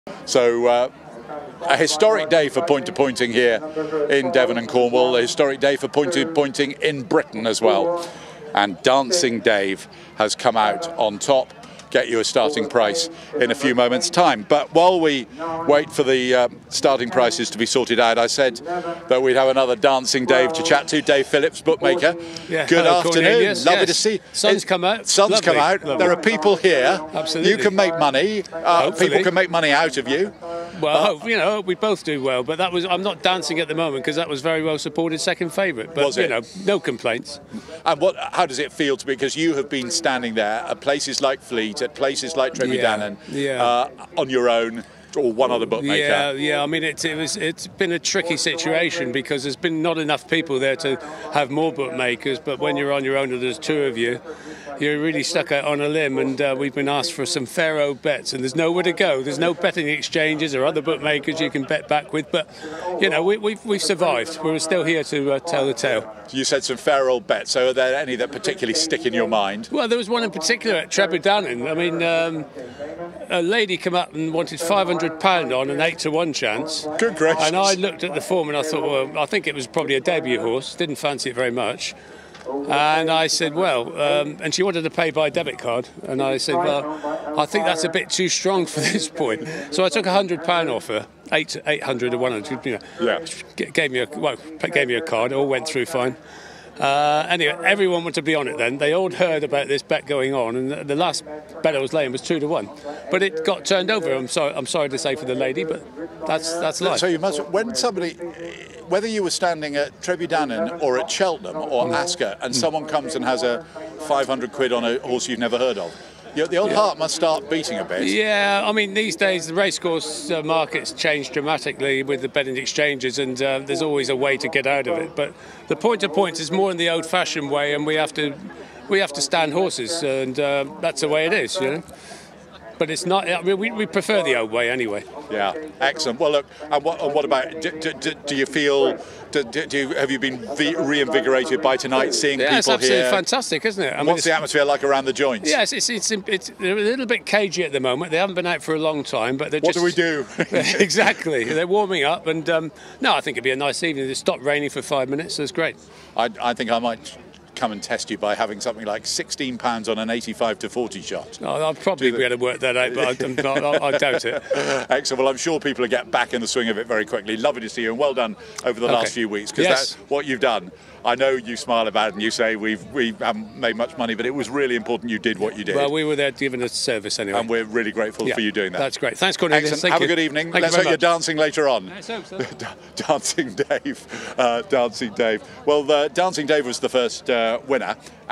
Bookmaker David Phillips talks to Cornelius Lysaght about the return of spectators at Bratton Down
GRANVILLE’S BETTING GOSSIP – DULVERTON WEST 17TH MAY

There was more of a real point-to-pointing atmosphere at Bratton Down for the Dulverton West meeting on Monday with the crowds returning on a very pleasant evening.

Eight familiar looking Devon & Cornwall bookmakers signalled a hint of normality, but sadly Mike Bolitho was not amongst them. Mike, whose board always showed fair odds, traded under the banner of his late father Roy, but had sadly died a few days ago. He had stood last weekend at the Eggesford’s Upcott Cross meeting with two of his colleagues. Sincere condolences go to Mike’s partner, family and friends from all pointing colleagues.

Edabean (5-4 to evens fav) was a surprisingly short price in the opening Restricted, considering that she had only won a Mares race. Maybe it was due to the in-form connections. To be fair, she didn’t get the best of runs in some scrimmaging up the hill before finishing fourth. The eventual winner Dancing Dave (solid 3’s) was only fifth jumping the last, but stayed on really well on the inside under a good Chloe Emsley ride. The runner up was 20-1 outsider Eole D’un Regard who last won two years ago. He had shown little since, but ran really well here under young Barnaby Treneer. Minella Beag (6’s to 7’s), from the Ed Walker yard, like the winner, came from the back to finish third. He had been running under rules and warrants a look next time.

It was time to get my wallet out in the 12-runner Maiden. Leslie Jefford’s Businessman (13-2) has been on my watch list for some time and looked real each way value. He led everywhere except where it matters as Jimmy Snipe (33-1) stuck his head in front on the line to win by a “nose” (not an official point-to-point distance- should be a short head). The winner, completing for the first time after three runs, is out of the prolific Sea Snipe (won 17 points) and was the first training success for Joe Tickle’s partner Danielle Kenealy. Arctic Milly (9-4 fav) always gets placed and did so again. Nuova Vita was backed from 4’s to 5-2 on the strength of his promising Flete debut, but was never really travelling after an early blunder.

Rolling Dylan (5-2 to 5-4 fav) once had a BHA rating of 143 and now trained by Laura Parker, ran in the colours of the Conditions race sponsor Philip Hobbs, his former trainer. Another useful ex-NH horse Horatio Hornblower (9’s out to 12’s) was making his pointing debut under a stiff penalty and ran well under his novice rider. I missed the 7-2 (returned 5-2) about Minim Mouse and you can guess what happened. Yes, Mini won by a squeak – yet another dreaded “nose” verdict. The runner up Coningsby had no friends in the ring (3’s out to 6’s) but ran up the hill to nearly catch the mouse.

Proven stayer Know the Score (11-10 to 4-5), trained by Leslie Jefford, found the four miles Mixed Open easier than his recent Cheltenham consignment, but was all out under the talented Fly McIntyre to hold Imogen’s Thunder (5’s to 7’s), given a grand ride by Charlie Sprake.

Dr Time (5-4 to 4-6) was all the rage for the 10-runner PPORA race to set up his hat trick under James King, but an early mistake did not help his cause. He was struggling after the ditch allowing the smooth travelling Wotzizname (10’s to 7’s) to hold the running on Skylander (7-2 out to 5’s). The young jockeys on both first and second, Megan Bevan and Otis Morgan, each came out with much credit.

The getting out stakes was a bit more predictable. The Yeats gelding Largy Mountain (5-4 / 6-5 fav), ridden by champion jockey elect James King, completed his English hat trick in good style (he was hopeless in Irish points and has taken time to get his act together), pulling away from French Piece (3’s to 5-2) after the latter had bungled the last. Third place Awesome Tunes (5’s to 4’s) kept jumping to the left but was not disgraced.